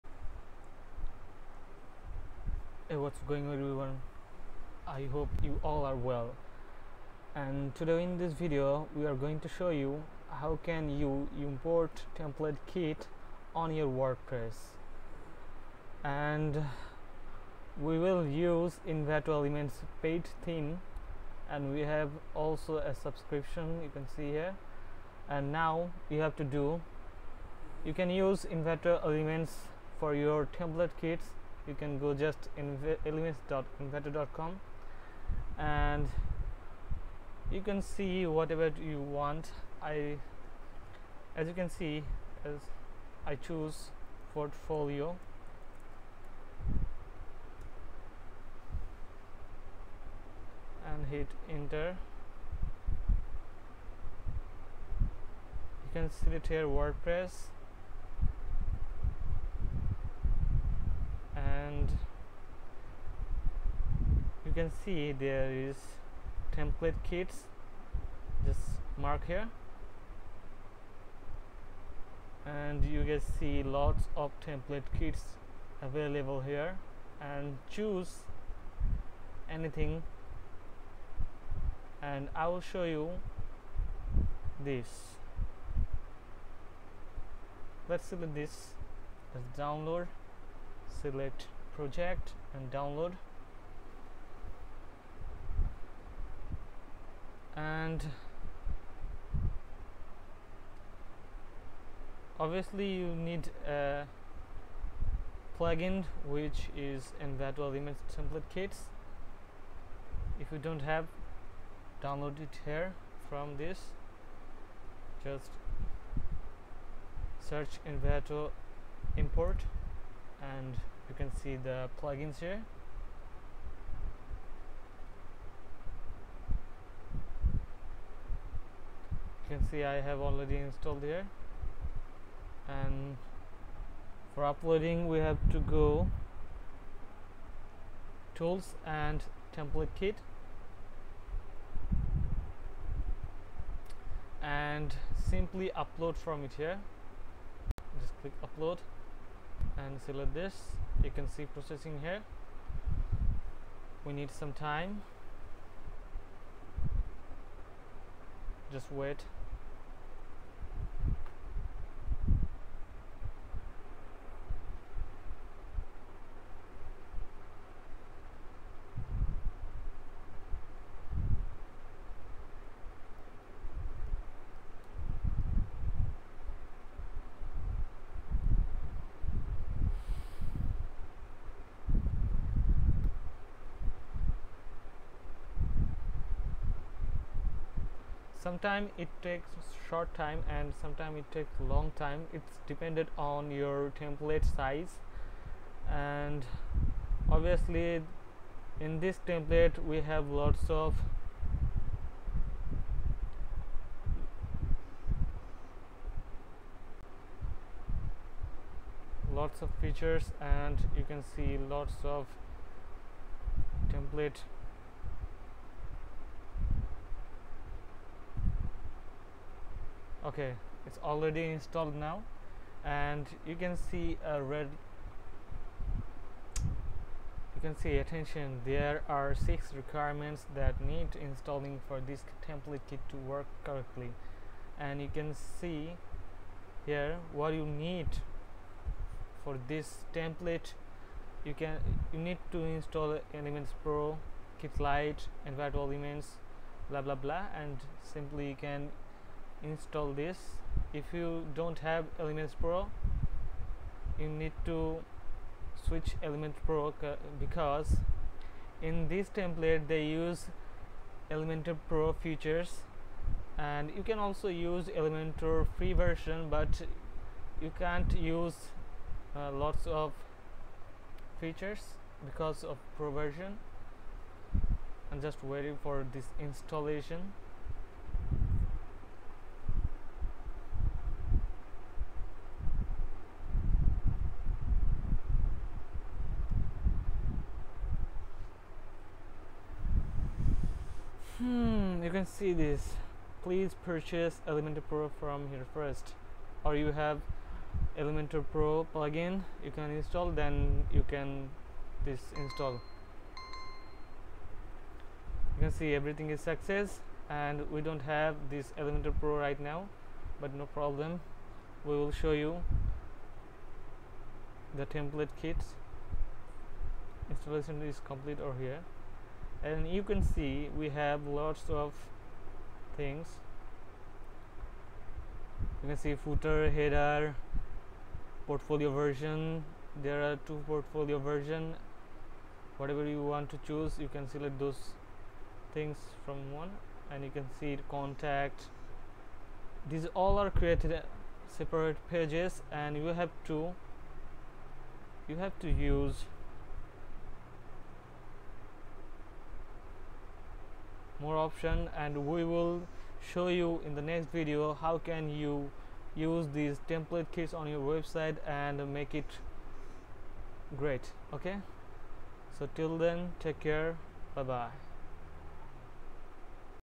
How to import Envato Elements Template kits on WordPress 2023 | part 2
In this video we showed you how can you install or import your templates to your wordpress website professionally without any problem.
Best Microphone
Best Keyboard
Best Mouse